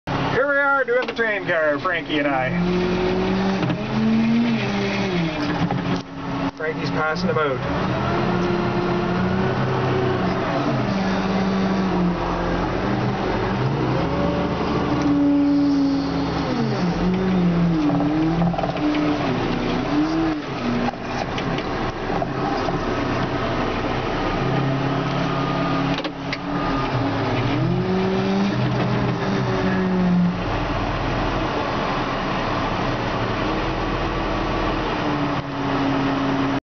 Unloading the Boxcar
The little forklift hands the bundles out to my big forklift